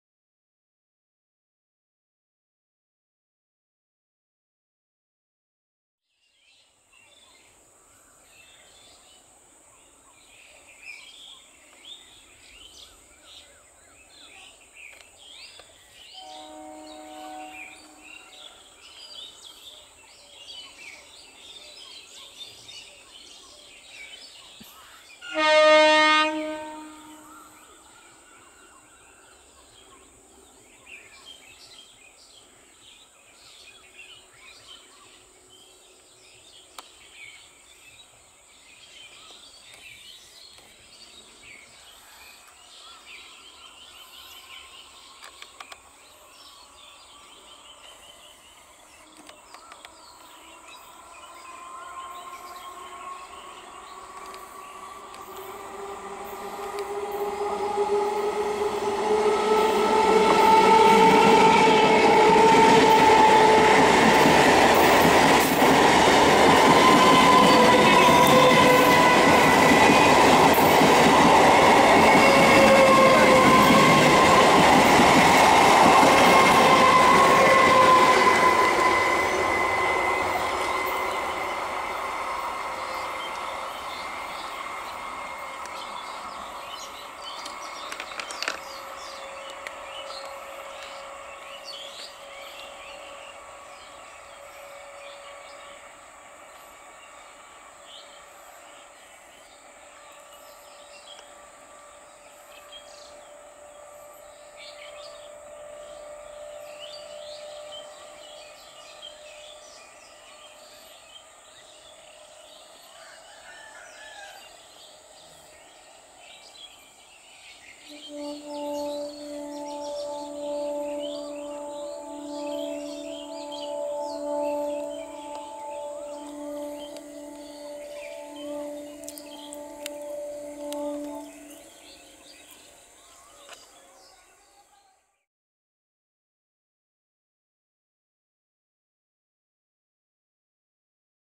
37748/Down Katwa-Bandel (EMU) Local || Acculturing System(Ready pick Up)|| Indian (Eastern) Railway
37748/Down Katwa-Bandel (EMU) Local Train Departing Balagarh Railway Station || Acculturing System(Ready pick  Up)|| Indian (Eastern) Railway

Video Location: Balagarh Railway Station